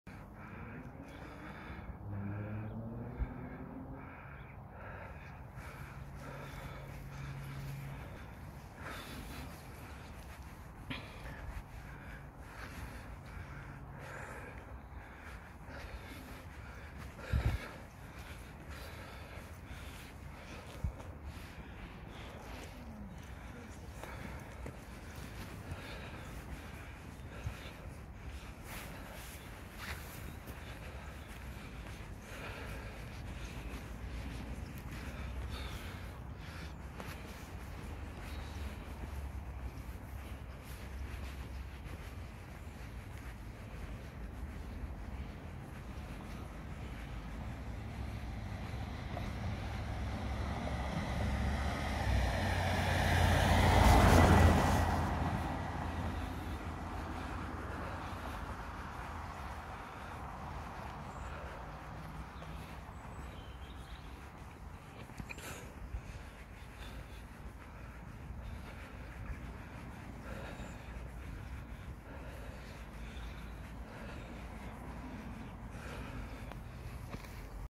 Evidence or not?
All I can do is post these clips for you as a benchmark for you to monitor whats happening to your area. Take your own videos to play spot the difference months from now, while being accosted with these chemicals, or find a decent copper to determine its all over my house and garden so it can be stopped now and you won't have to bother.